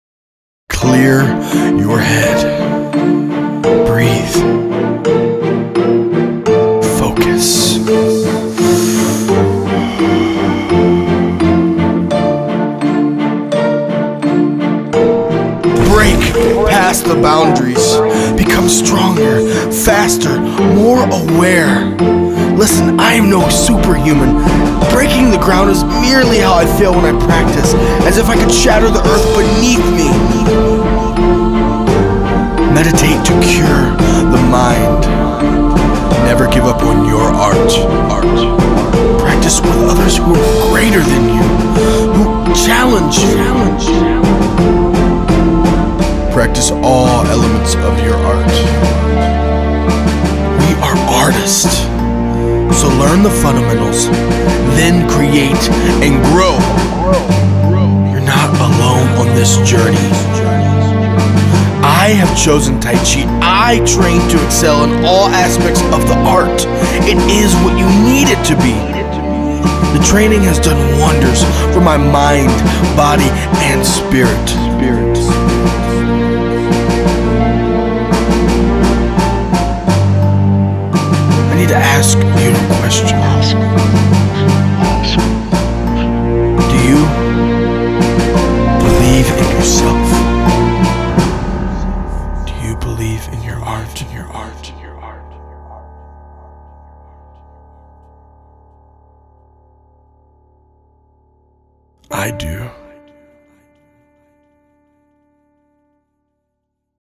Sifu Nicholas Alsup - "Do you Believe."
Watch ENTIRE video and ask yourself...
Do you believe in yourself? Do you believe in what your doing?

I Do... "I Sifu Nicholas Alsup believe in my ART! Tai Chi!"

We get one chance...
We are constantly moving forward, take this momentum
and make a difference in your life; do something to strengthen your body, mind and spirit!

justdoit don't let anyone hold you down, don't let anyone hurt you...

Video Cred: Alex White
Audio Engineer: Sybil Smith

help us SPREAD the word!
Tai Chi is amazing and it works!